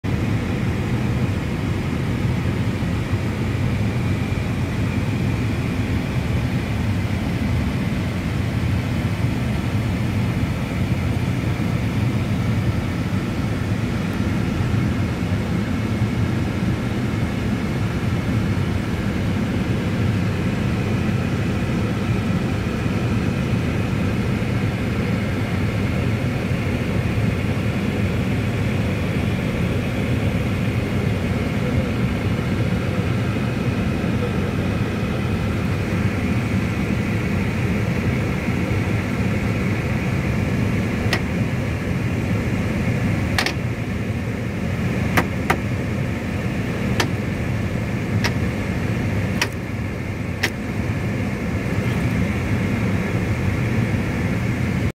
A320 THROTTLE LEVER POSITIONS
Hi All,
      Please have a watch of Airbus 320 throttle control unit or power lever we can say.

TOGA :TAKE OFF AND GO AROUND POWER(MAX POWER)

FLX/MCT:(FLEX TEMP TAKE OFF/MAXIMUM CONTINUOUS THRUST(INCASE OF SINGLE ENGINE)

CL : CLIMB

IDLE(0) : IDLE POWER(AT START)

REV IDLE (YELLOW LINE STARTS): THRUST REVERSER DEPLOYMENT WITH IDLE REVERSE POWER.

REV MAX(FULL) : THRUST REVERSER DEPLOYMENT WITH MAXIMUM REVERSE POWER.